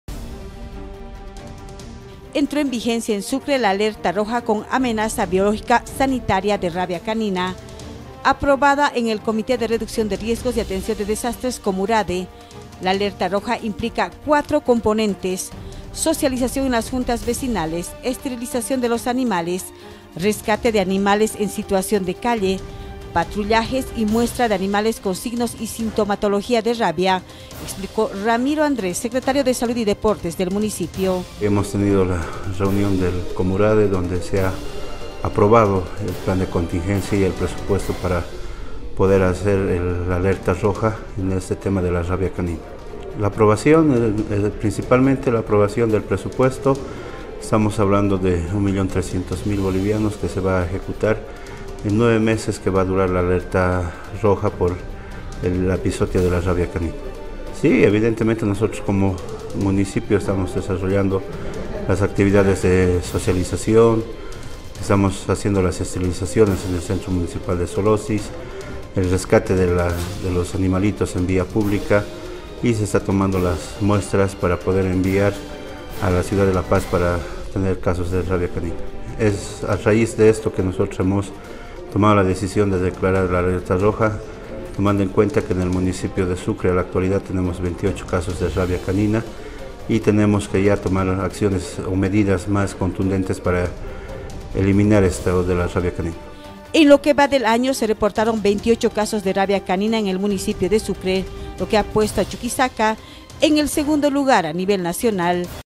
RIGE ALERTA ROJA POR RABIA CANINA EN EL MUNICIPIO
Recordemos que los animales actúan como portadores del virus de la rabia canina, el cual se propaga a través de la saliva después de una mordedura. Con la implementación de esta Alerta Roja, las autoridades buscan detener el avance de esta amenaza y proteger tanto la salud de la población como el bienestar de los animales.